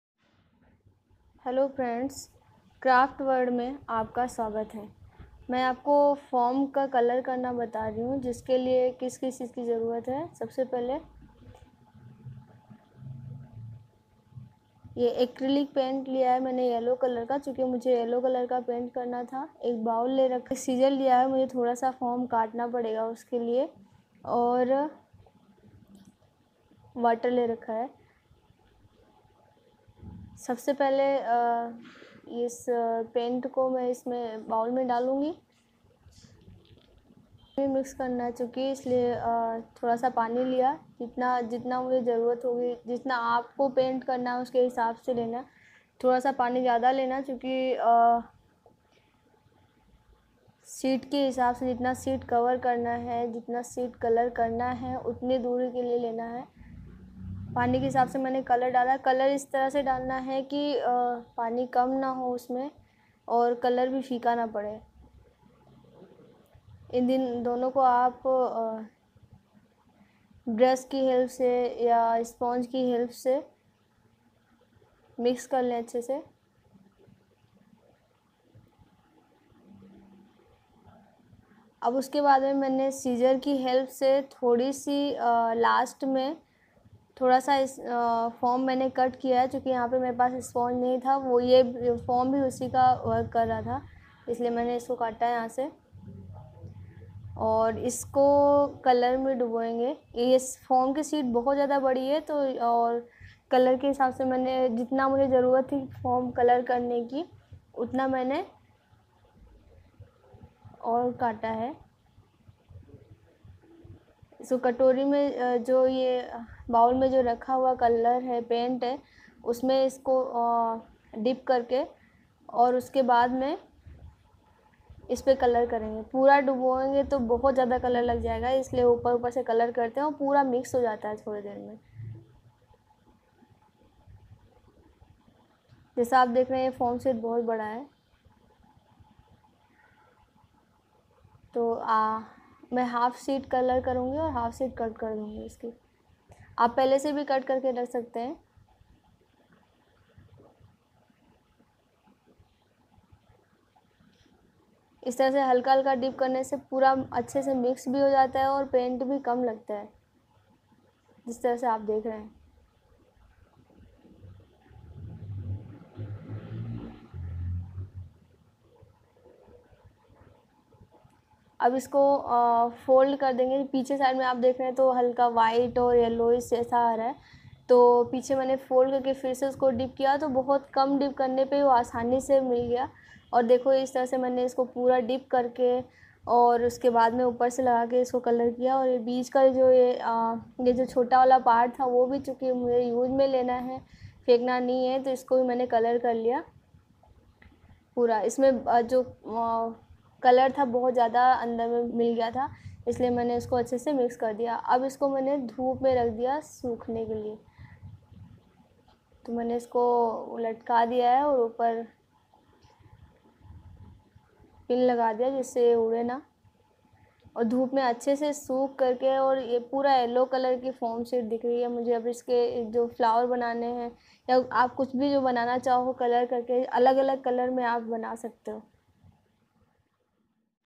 How to paint Foam sheet in Hindi // How to Color Foam Sheets at Home in Hindi
Here we will discuss about how to paint /color Foam sheet